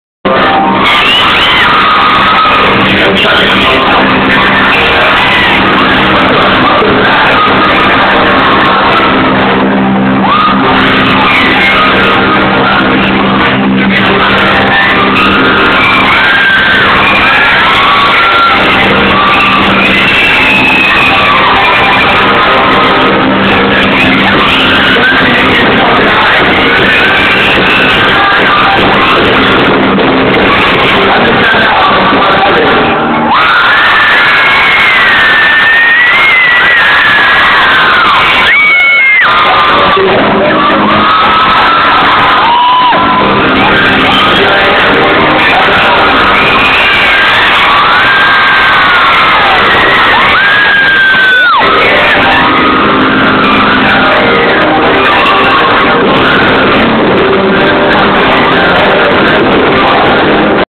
wiL francis gets gloved
31st march 2008. dublin academy.
wiL was saying something and someone threw a glove and he said 'what the fuck was that?' it was so funny seing his reaction!! that was anawesome gig!! i loved it!!